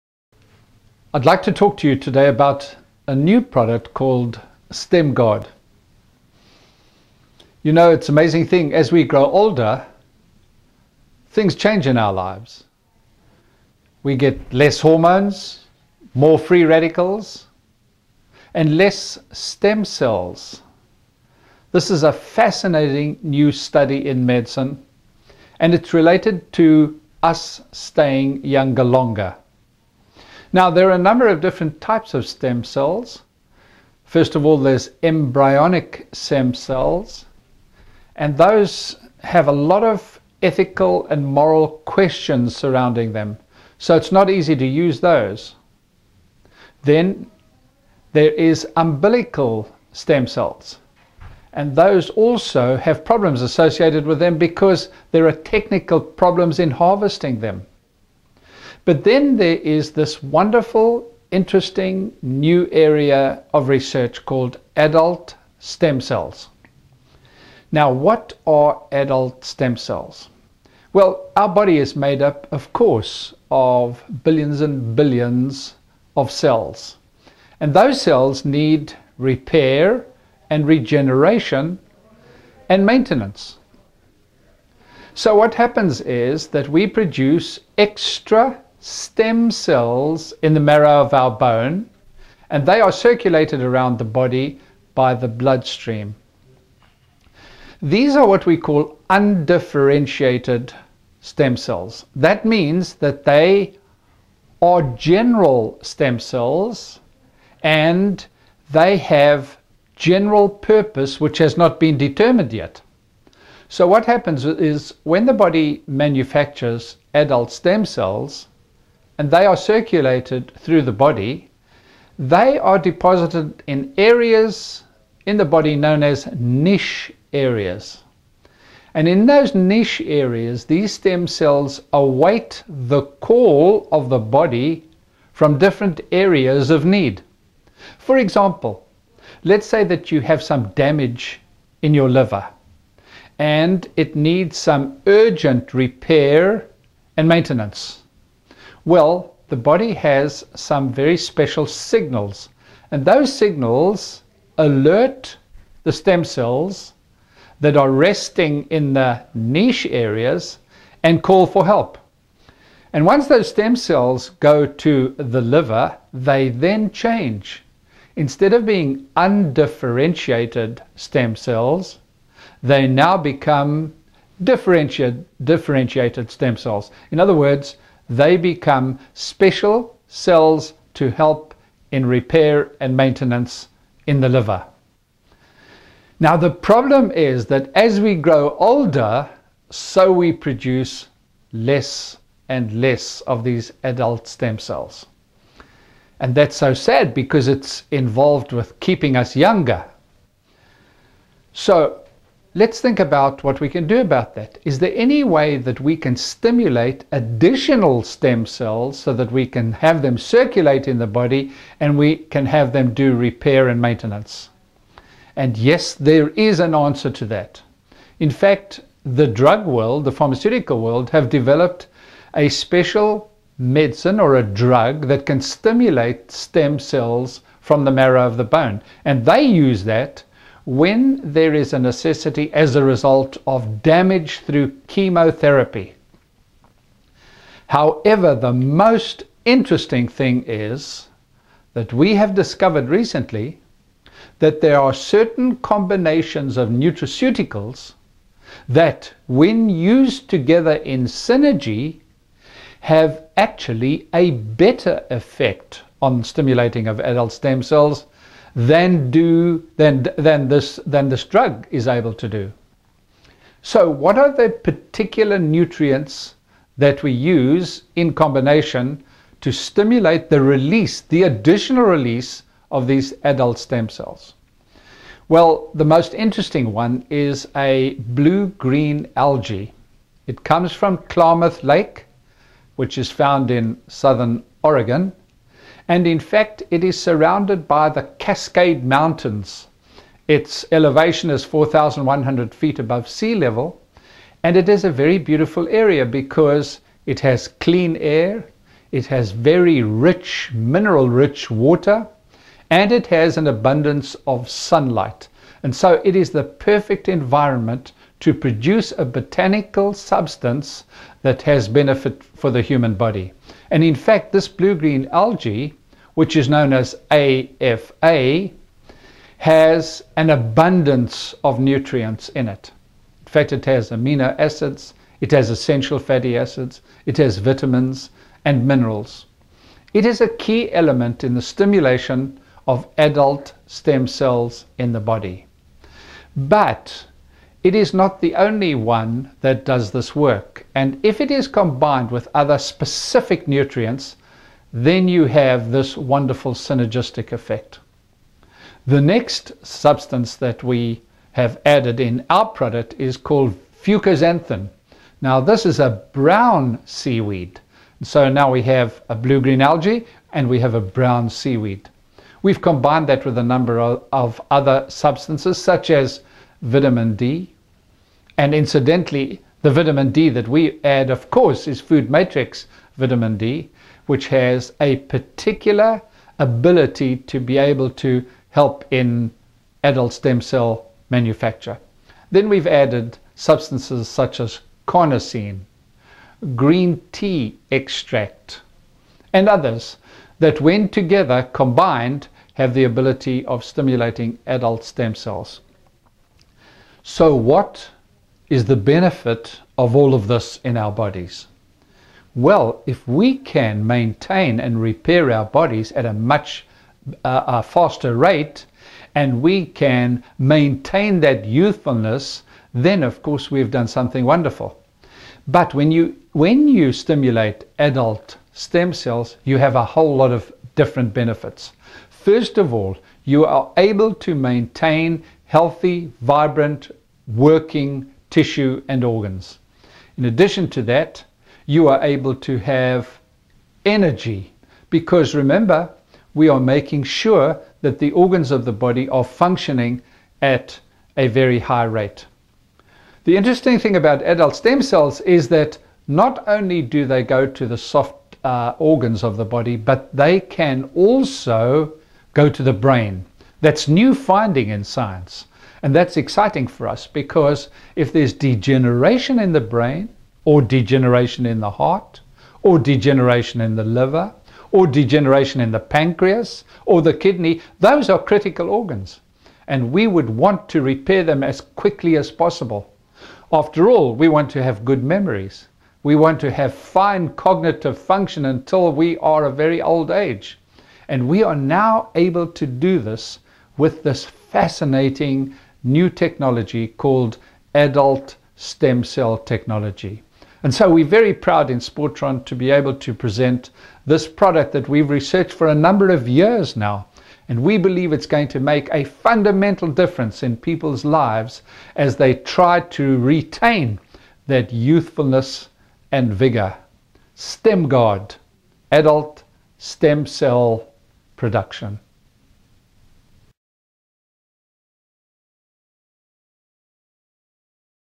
StemGard
Dr. Tomlinson discusses StemGard™, the only FoodMatrix® product that supports the natural release of adult stem cells.